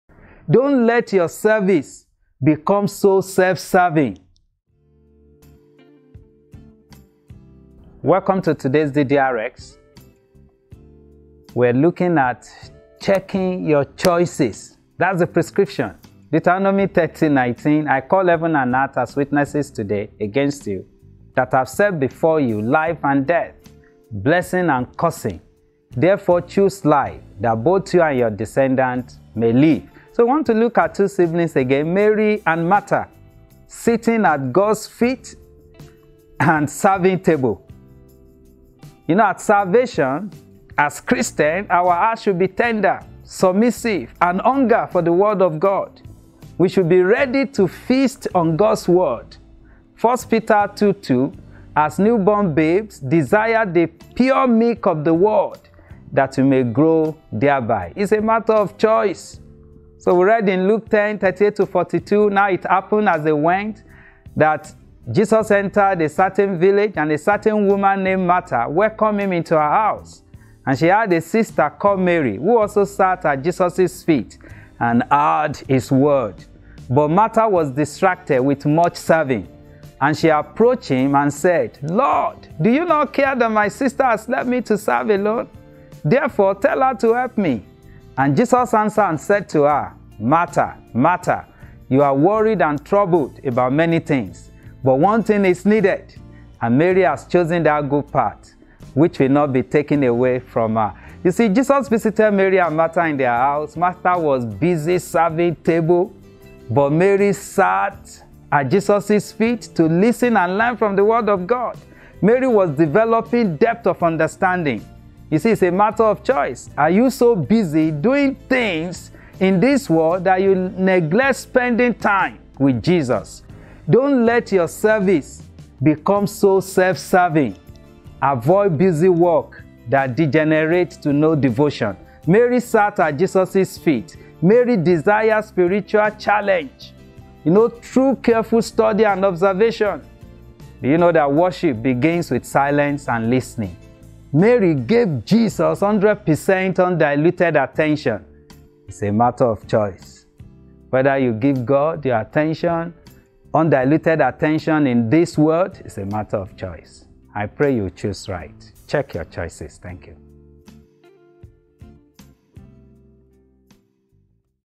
DDRxS24E07: Check Your Choices- Mary & Martha - Sitting at God’s Feet vs Serving Table
In today's DDRx, we explore the story of Mary and Martha. Don't let your service become so self serving. Give God your undiluted attention

Stay in Touch with Us!